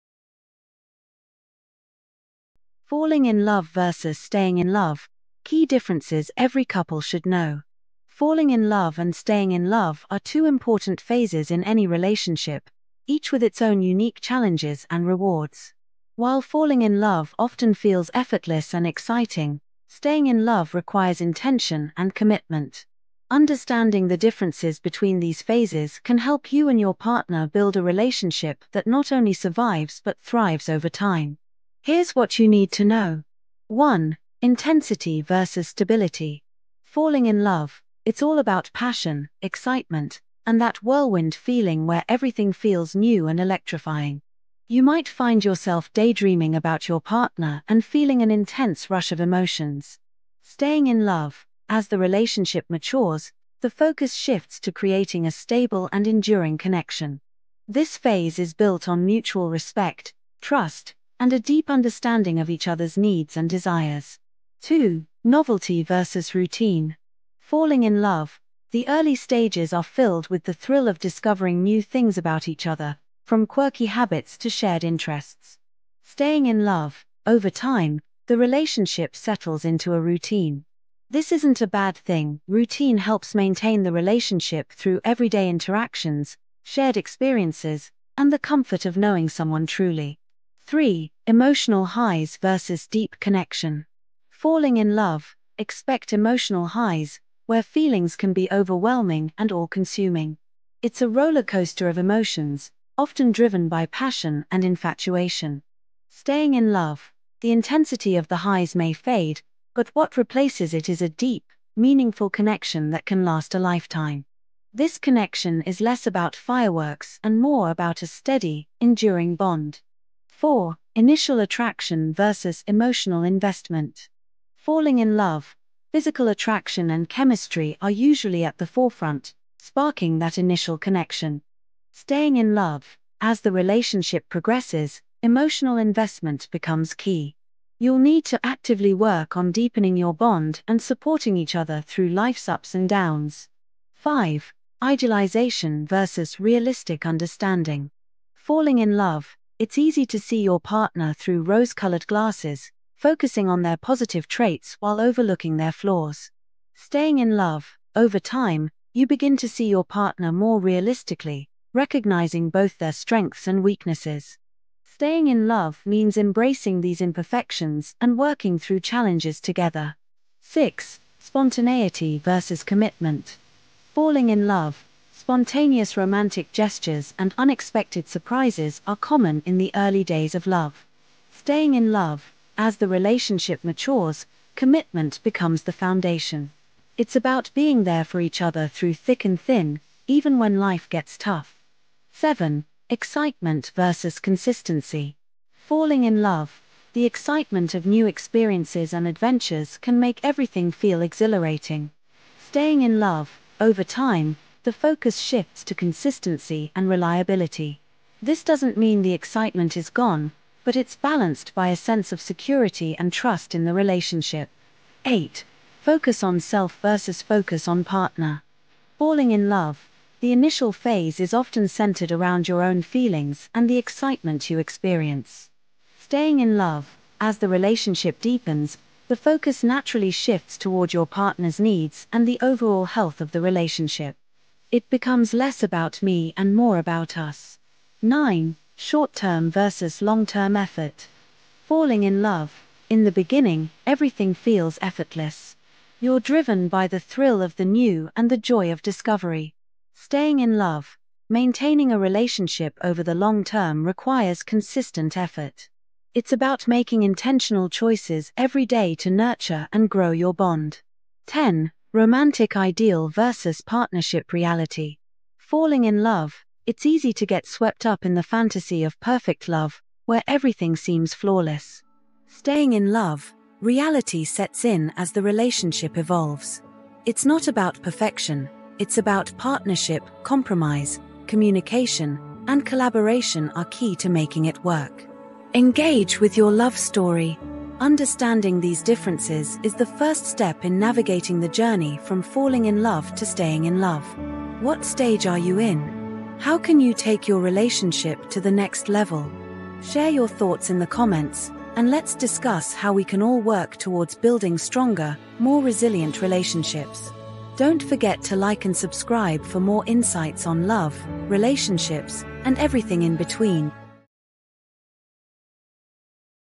Falling in vs staying in love
Falling in love vs staying in love

Falling in love and staying in love are two distinct phases in the journey of a relationship. Understanding these differences can help couples navigate their relationships more effectively and build a lasting, fulfilling partnership. Watch here ten key points that highlight the differences between falling in love and staying in love.